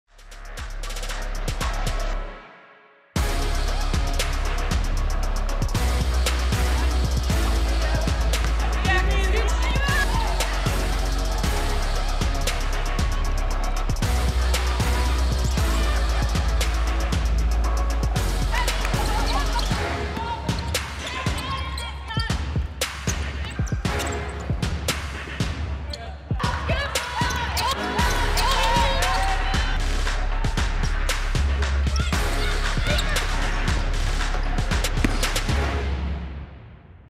Holy Cross Women's Basketball
Putting that work in with just one month away from the season opener at Illinois